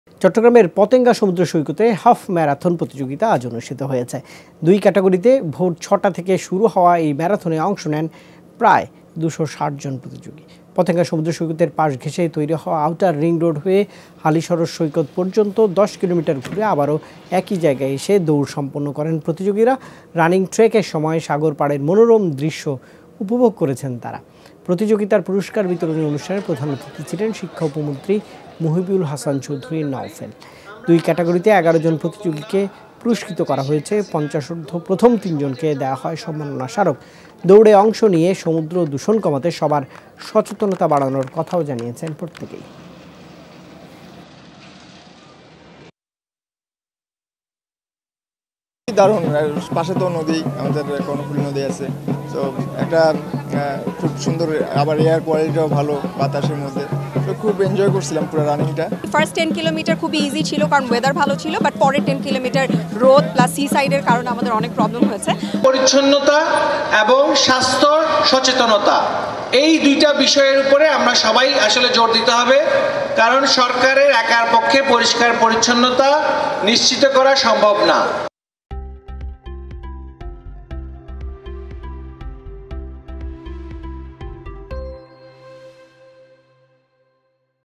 সমুদ্রের দূষণ কমানোর আহ্বানে পতেঙ্গায় হাফ ম্যারাথন | Marathon
চট্টগ্রামের পতেঙ্গা সমুদ্র সৈকতে হাফ ম্যারাথন প্রতিযোগিতা অনুষ্ঠিত হয়েছে। দুই ক্যাটাগরিতে ভোর ৬ টা থেকে শুরু হওয়া এই ম্যারাথনে অংশ নেন ২৬০ জন।